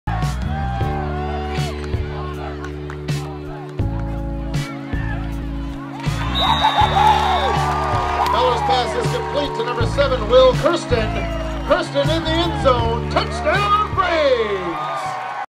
Kersten catches a TD pass from Bellar. 8:24 left, Battle Creek 35, Oakland-Craig 6.
Will Kersten's second touchdown catch of the night helped lead Battle Creek to a 41-6 win over Oakland-Craig in Class C2 football on Friday, Sept. 5, 2014, in Battle Creek.